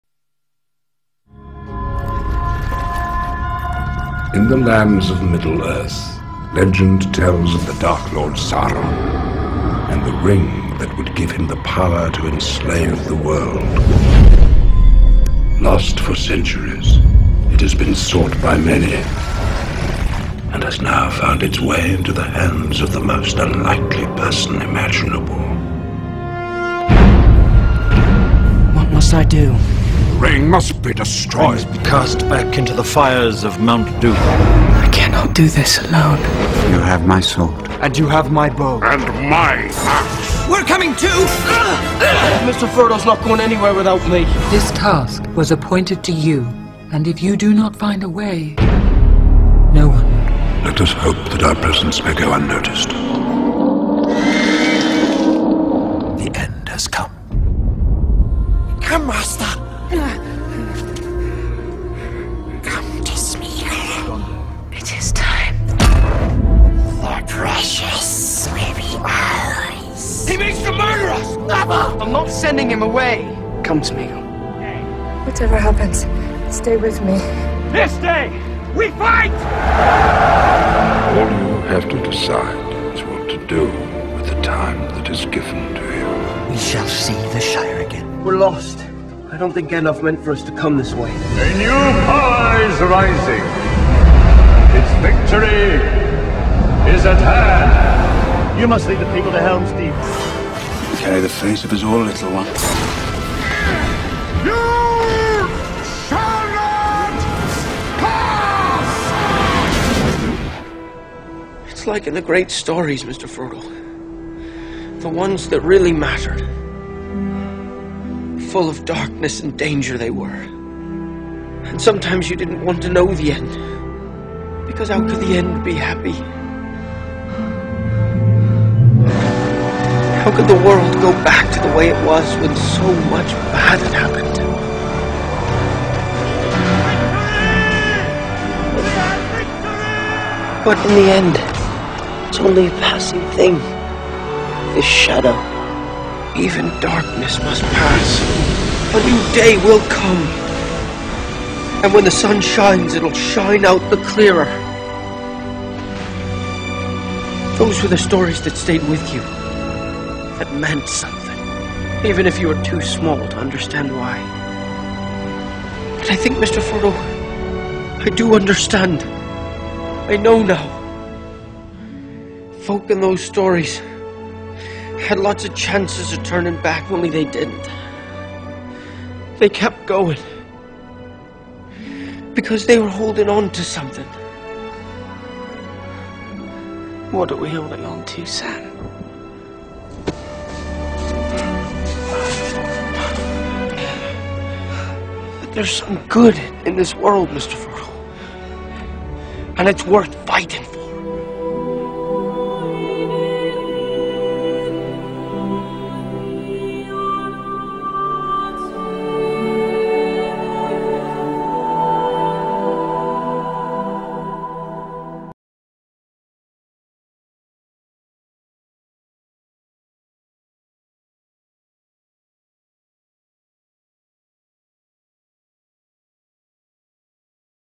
Lord of the Rings Wedding Spoof
My sister got married and I had fun making a spoof of Lord of the Rings to their wedding. Kinda corny, but i thought it was fun!